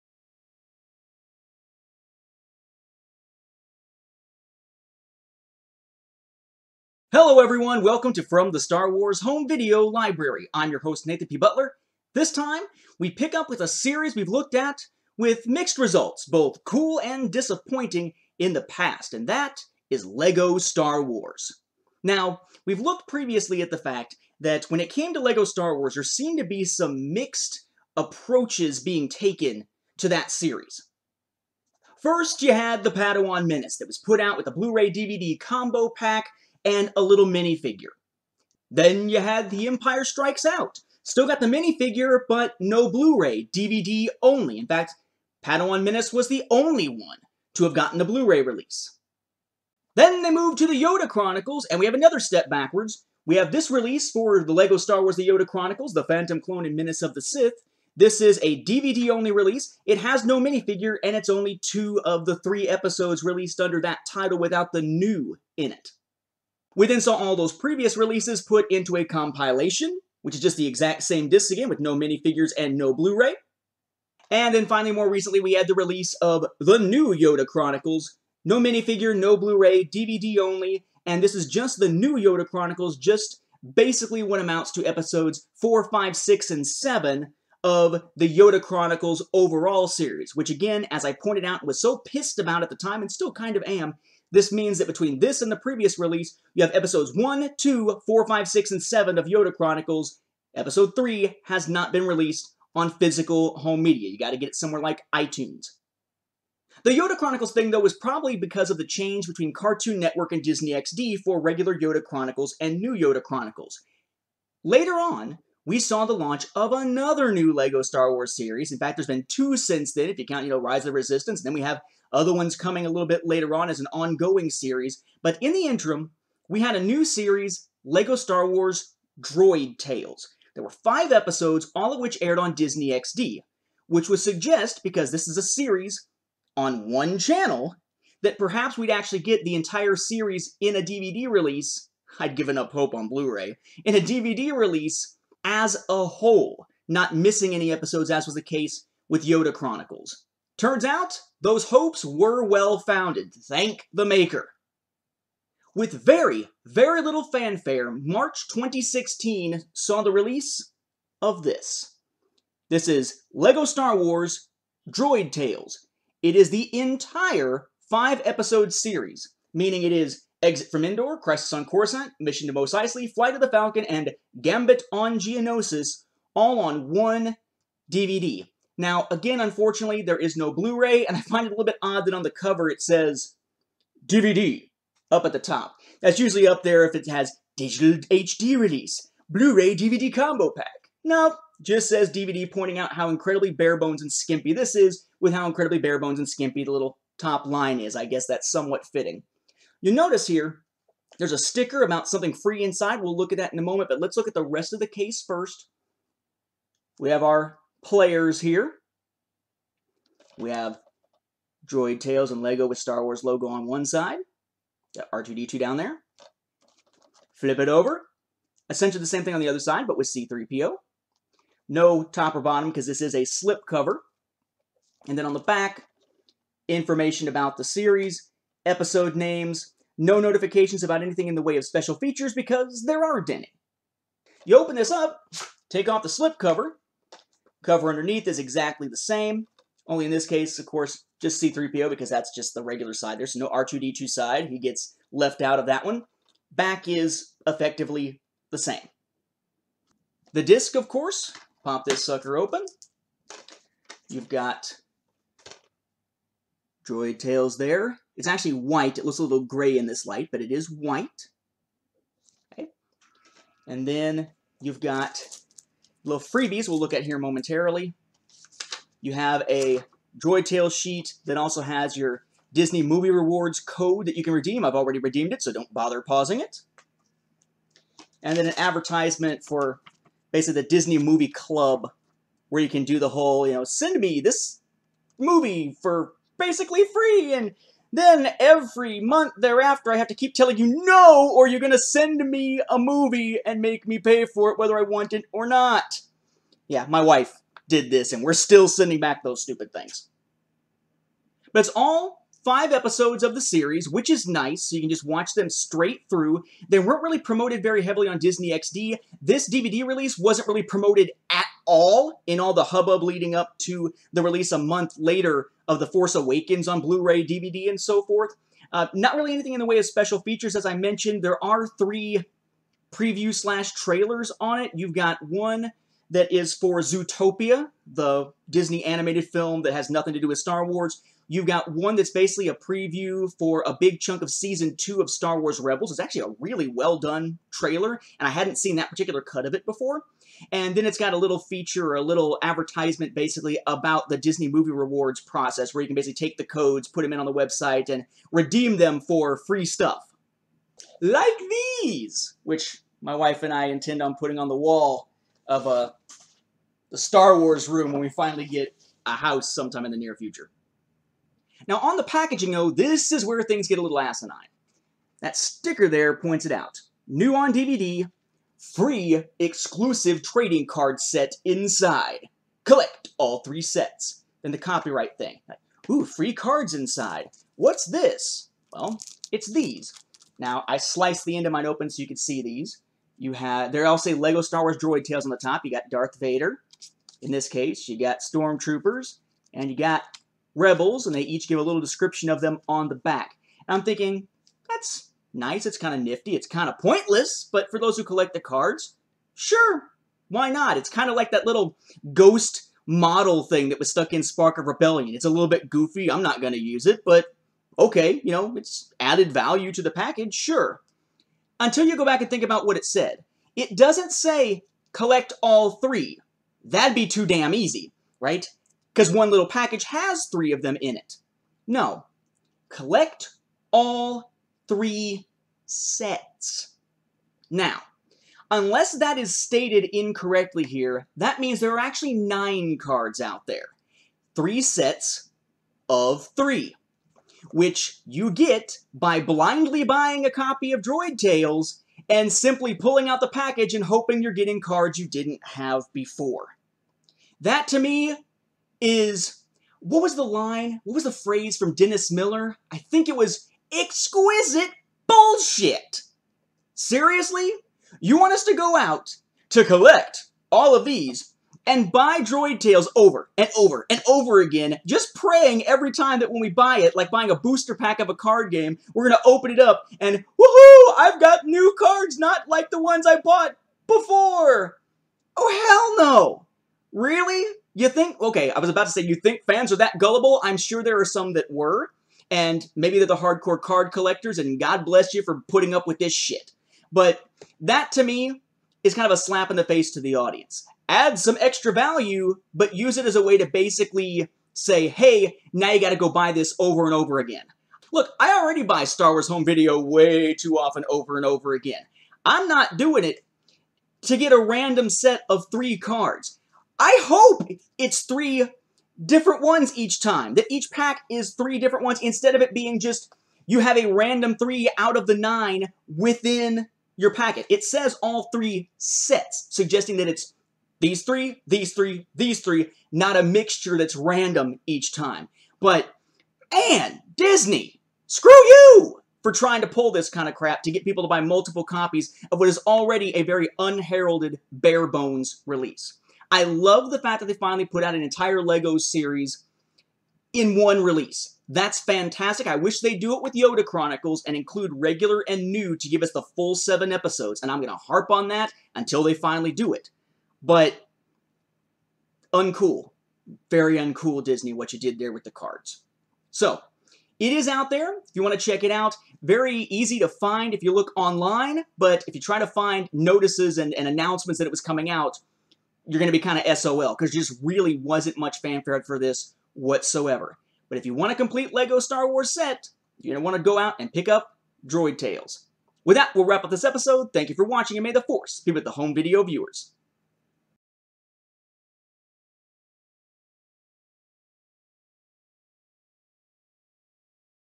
From the Star Wars Home Video Library #67: LEGO Star Wars: Droid Tales
This episode of From the Star Wars Home Video Library focuses upon the DVD release of LEGO Star Wars: Droid Tales (2016).